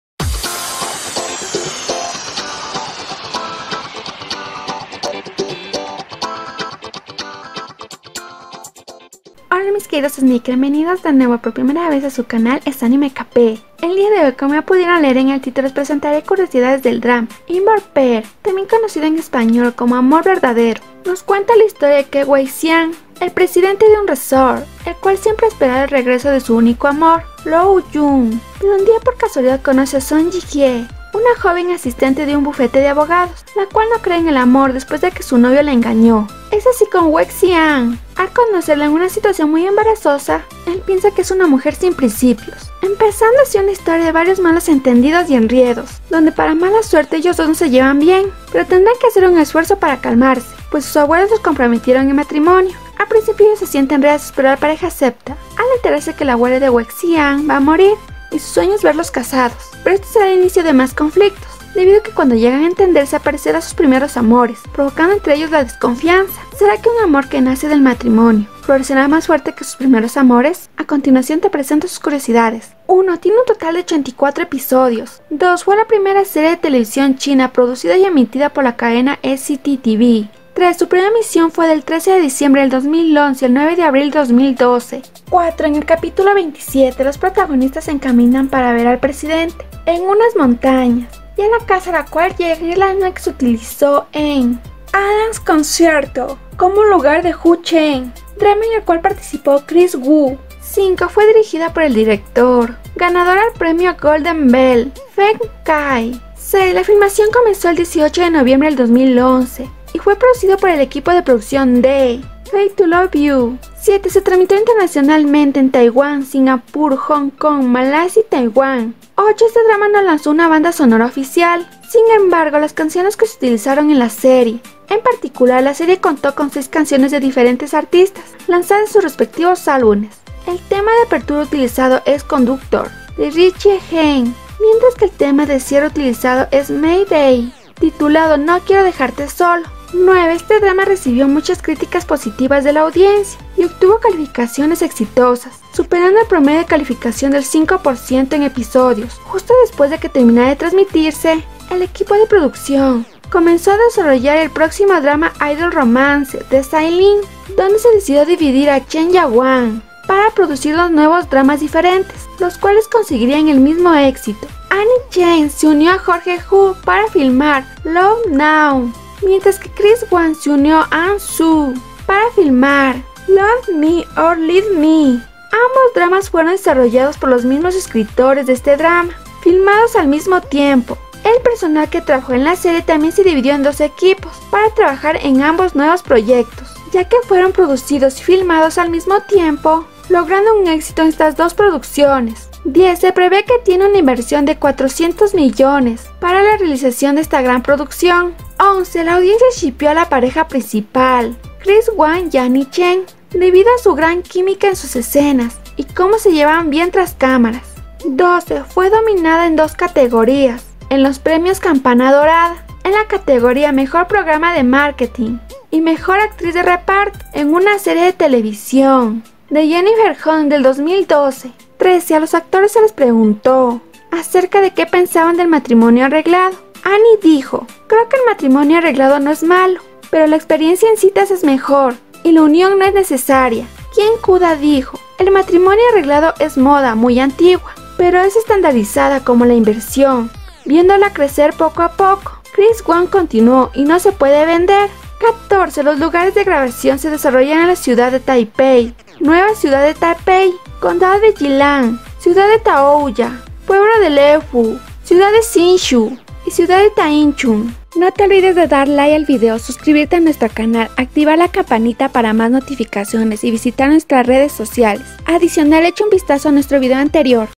CURIOSIDADES DEL DRAMA 😍 INBORN PAIR || AMOR VERDADERO 😍
Hola mis queridos seguidores el día de hoy les presentare curiosidades del drama Inborn Pair. Nos cuenta la historia de Ke Wei Xiang y Song Yi Jie, dos personas que por casualidades de la vida se terminan casándose debido a un arreglo entre sus abuelos.

Creditos:
© Licencia: